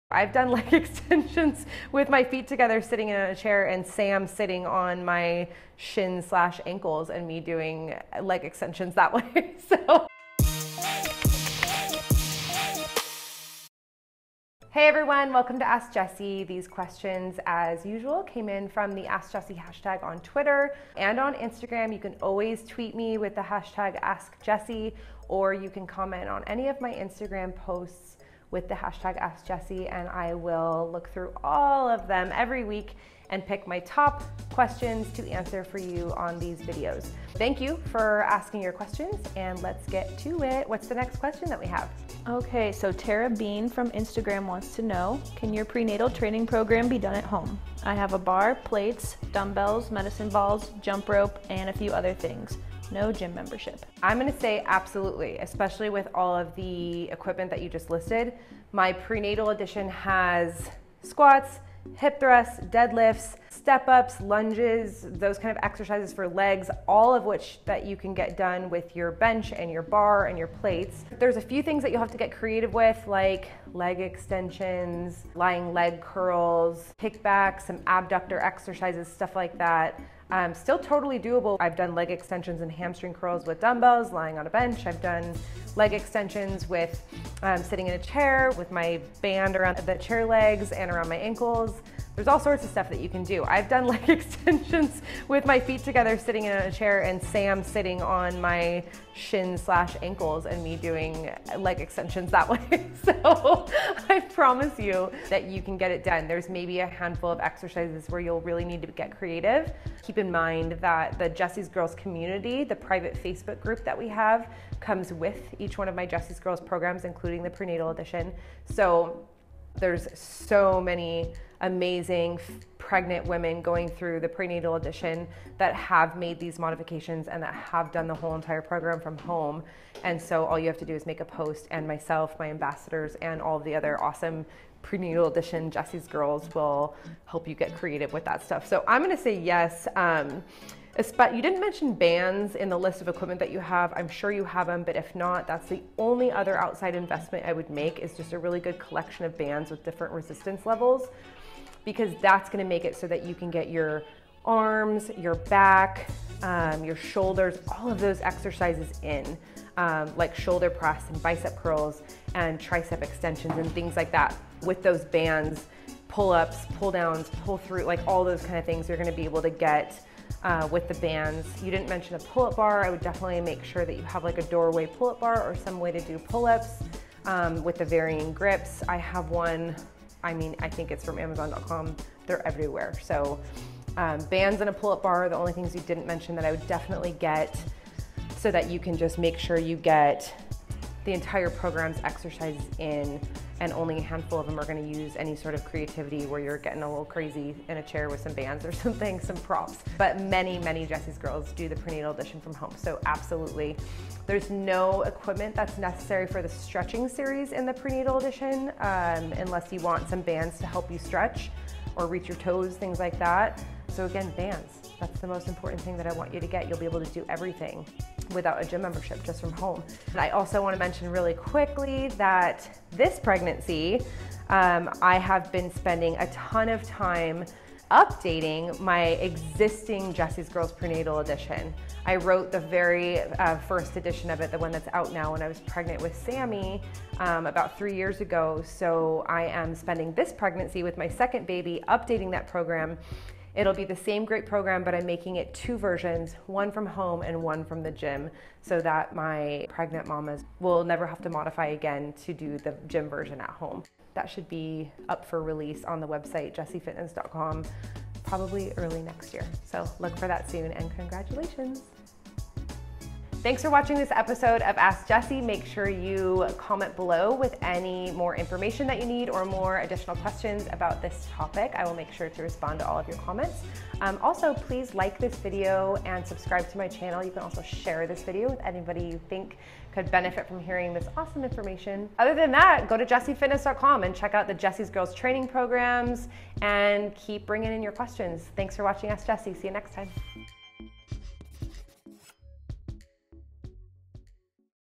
How to Workout From Home While Pregnant | Ask Jessie
Can the Jessie's Girls Prenatal Edition be done from home? Yes, it can! The prenatal edition is full of squats, hip thrusts, dead lifts, step ups, lunges, leg extensions and leg curls, and all these can be done at home with some creativity. I do leg extensions sitting in a chair and Sammie sits on my shins/ankles. I also suggest you use an at-home pull up bar and invest in good bands for arms, back and shoulder exercises. I promise you - with a little creativity, you can figure out how to workout from home while pregnant.

There's a handful of exercises where you may need to get creative, but stay in touch with the Jessie's Girls community for modifications and ideas. On that note, I'm in the process of updating my prenatal edition with two awesome versions - one from home and one from the gym. My pregnant mommas will never have to modify again! Watch out for those early next year.

Order B-Elite Fuel meals when you’re on the go!